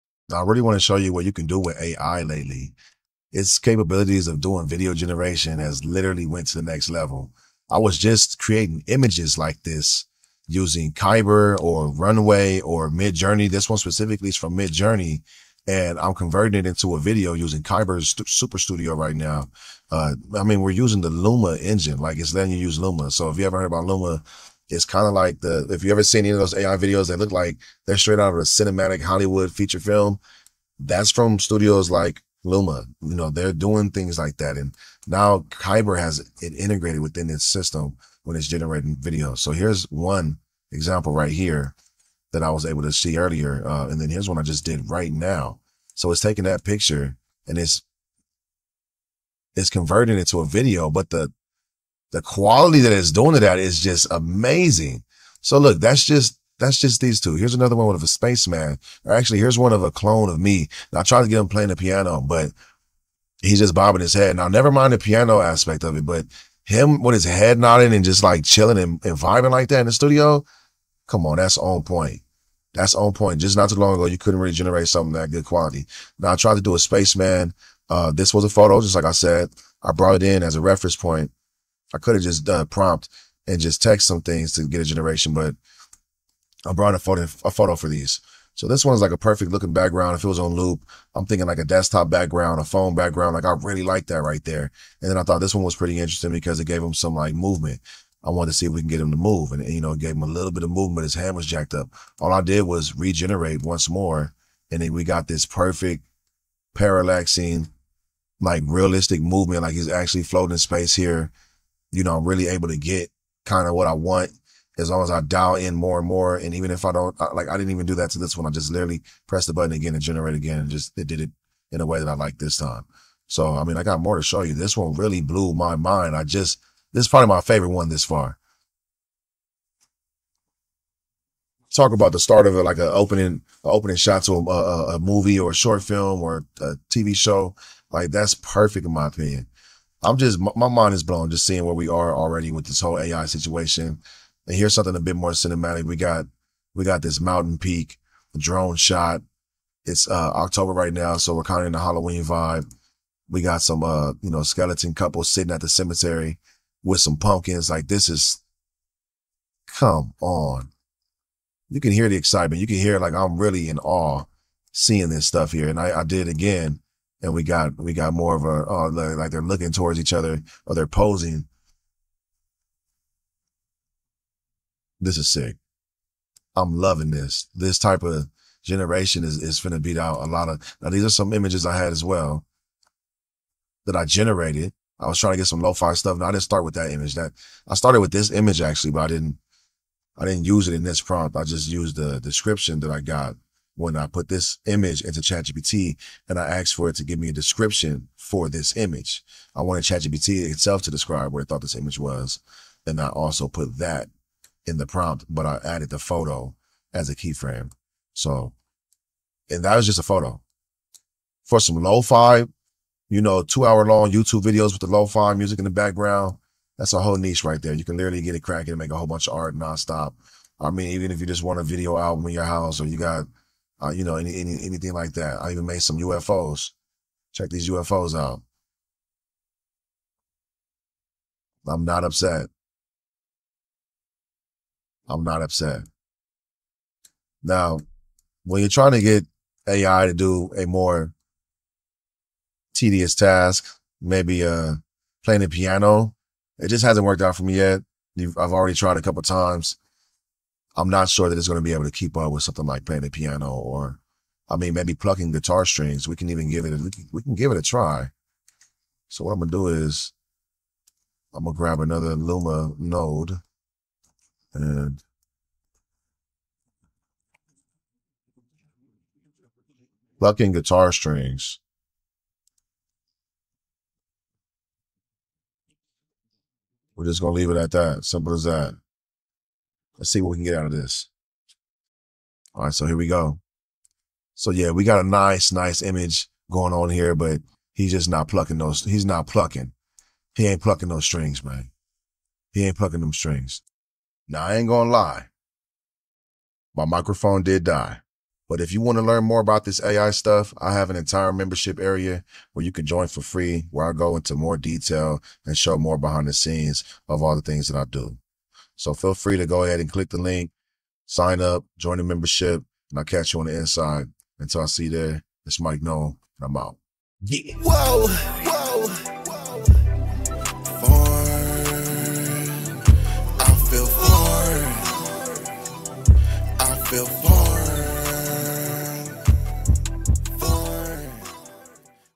Reaching Ai's Limits With Kaiber Superstudio, MidJourney, and Luma
Unlock the next level of creativity with AI! In this video, I dive deep into AI video generation with tools like Kaiber's Super Studio, MidJourney, and Luma. See how I convert static images into stunning cinematic videos and explore mind-blowing visuals in real-time. Want to learn how to master AI for your projects? I’ll walk you through the capabilities of these AI tools and show you how you can turn your art into mesmerizing videos.

🔗 Join my FREE membership to access exclusive tutorials and behind-the-scenes content

🎥 Key Highlights:

Converting AI art into videos
How to use Kaiber Super Studio
MidJourney, Luma, and other top AI tools
Creating cinematic video content
Real-time examples & walkthroughs